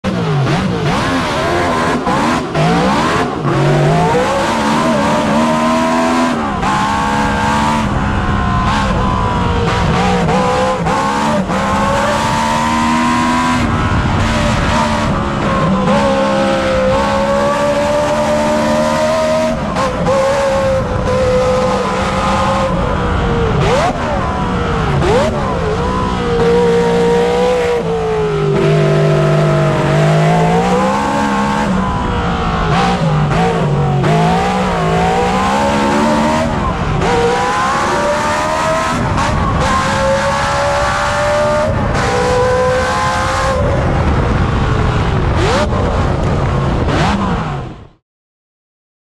Amazing BRM V16 onboard and on full song at Shelsley Walsh Speed Hill Climb.
On board with the continuation car of the BRM V16 1.5 litre Supercharge at the Classic Nostalgia weekend at Shelsley Walsh in July 2022.

Driven by Rick Hall, this is the epitome of commitment, prowess and art all as one.